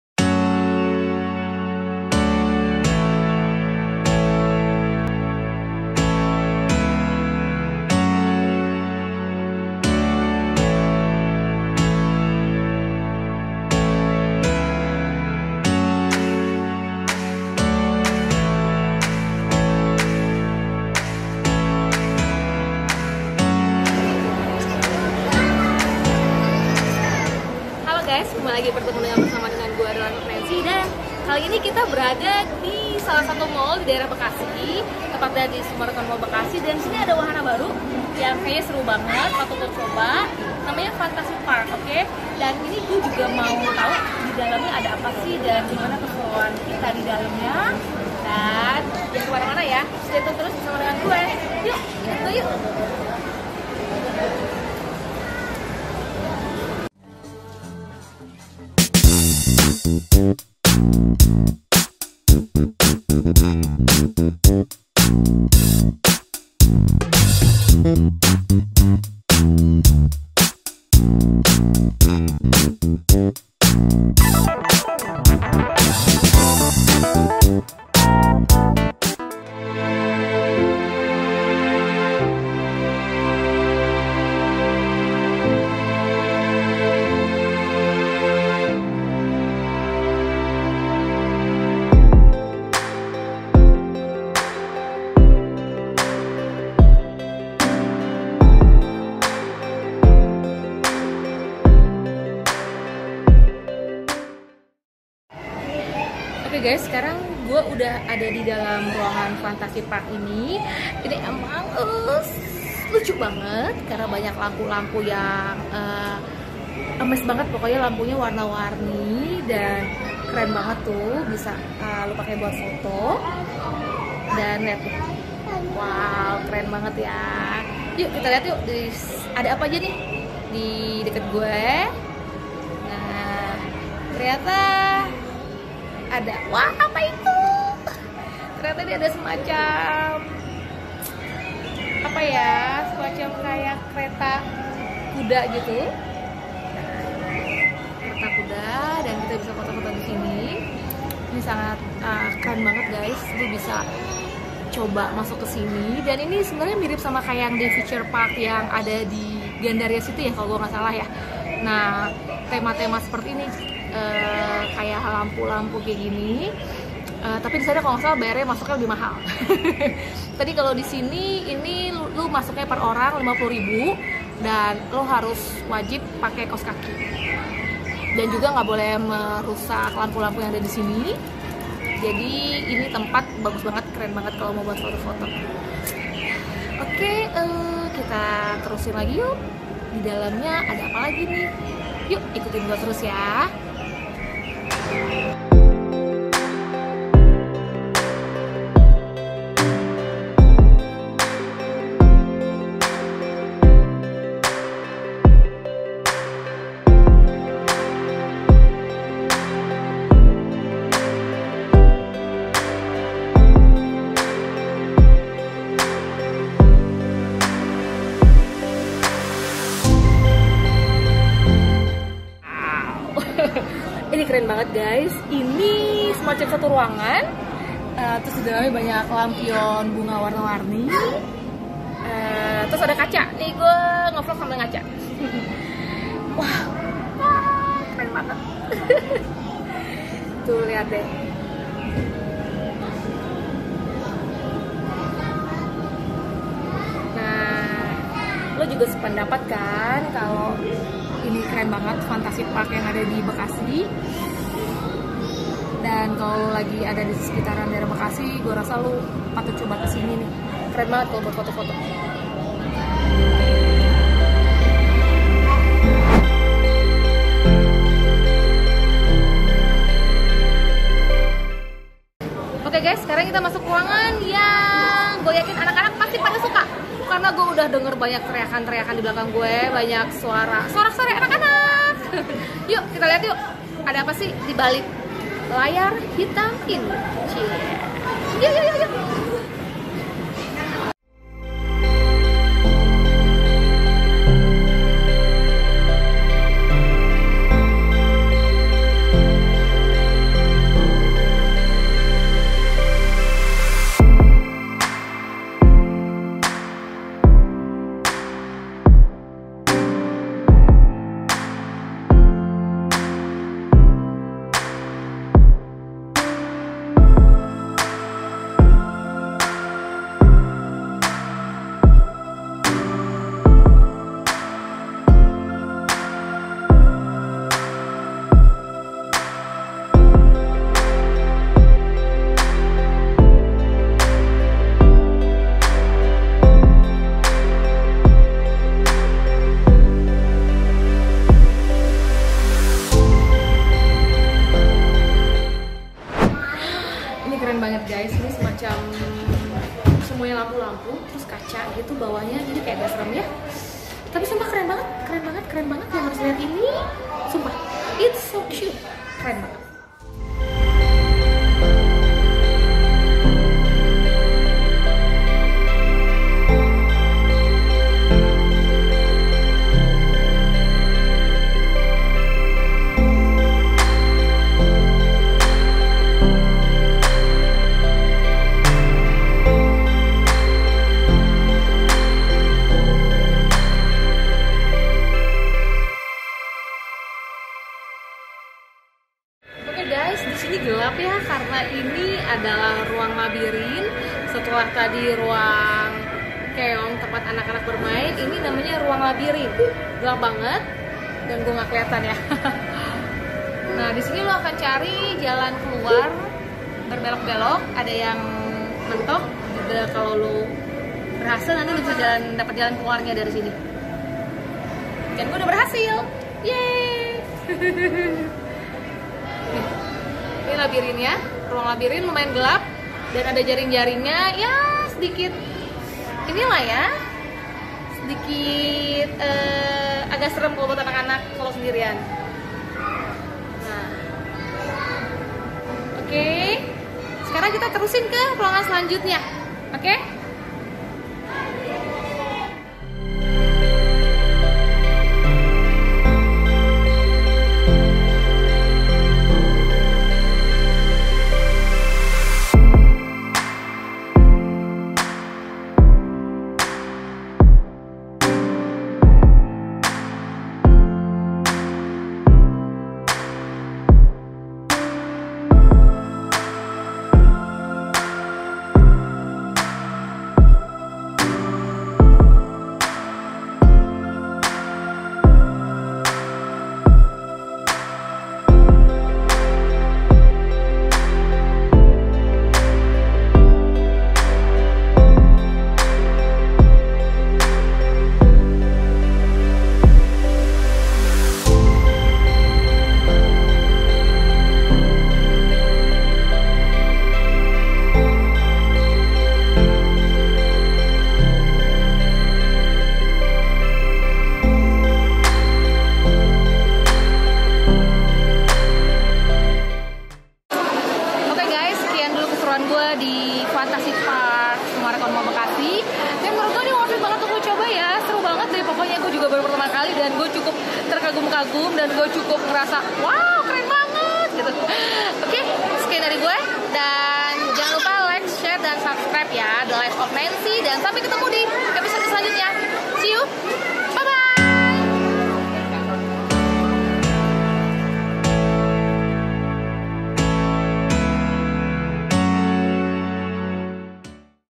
Fantasy Park Summarecon Mall Bekasi. Instagramable Banget!!
Fantasy Park
Summarecon Mall Bekasi Lt. 1

24 Oktober 2019 - Feb 2020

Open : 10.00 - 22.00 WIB

Fee : 50.000 /orang

Senin - Minggu

Wajib pakai kaos kaki. Bisa di beli di kasir seharga 10rb/pasang